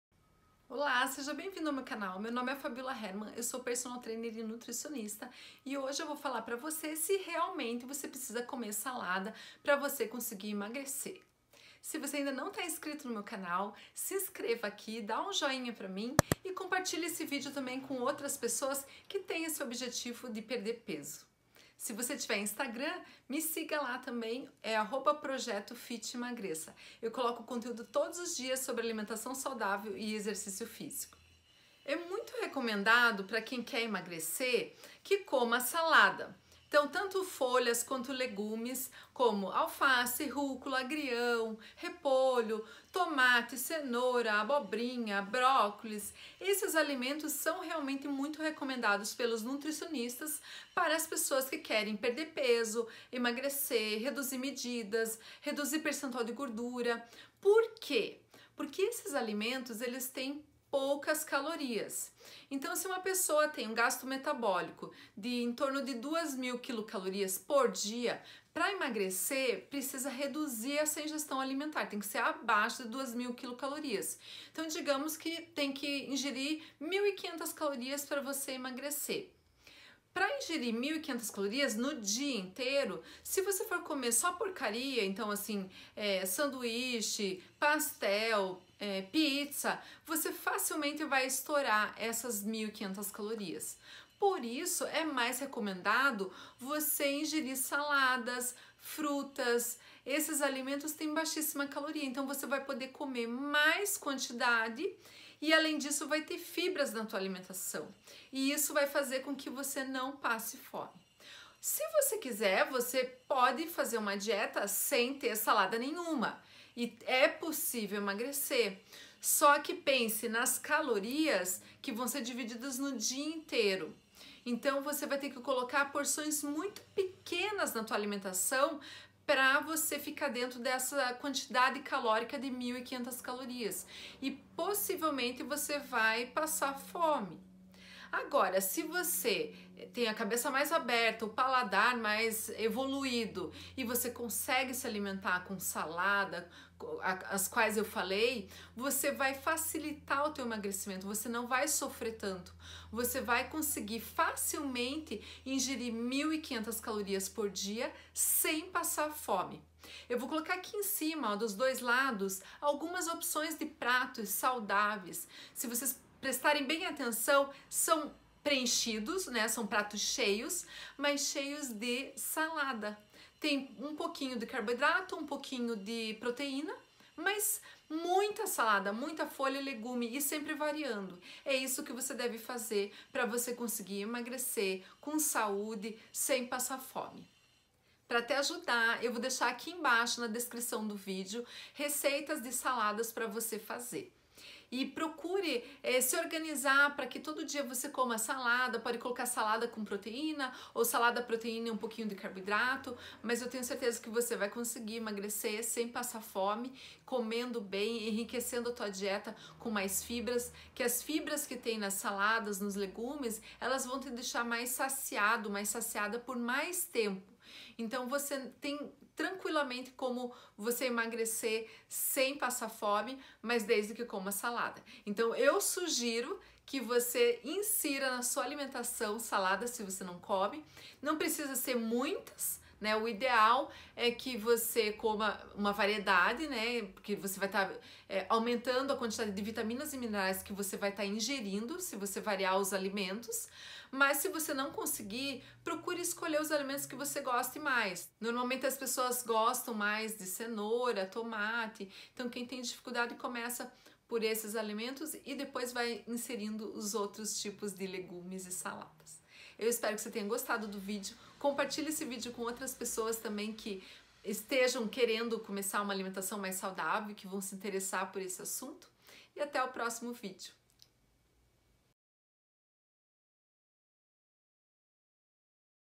PRECISA COMER SALADA PARA EMAGRECER? / Como perder peso rápido em saudável em 2021
Para emagrecer é necessário controlar as calorias que você ingere. Pode ser  uma dieta com salada ou sem. Mas é lógico que as escolhas dos alimentos irão interferir nas calorias e no emagrecimento. Se você quer perder peso prefira comer mais saladas, tanto folhas quanto legumes. Aqui embaixo vou colocar algumas receitas de saladas proteicas para te ajudar!

SALADA DE FRANGO COM RÚCULA
Azeite de oliva 1 colher de sopa
10 mini-cenouras picadas
1/2 xícara repolho roxo picado
1 xícara de rúcula
Frango em cubos 200g
2 colher de chá de sementes de girassol

SALADA DE ESPINAFRE COM AMENDOAS
1 1/2 xícara de espinafre
1/2 xícara de brócolis picado
1 tomate pequeno Roma, em cubos
3 ovos cozidos
1 colher de sopa de amêndoas fatiadas
Folhas de manjericão fresco
VIDEOS QUE POSSAM TE INTERESSAR:
CHÁS
Chá Desincha
Chá Hibisco com Canela e Gengibre Grings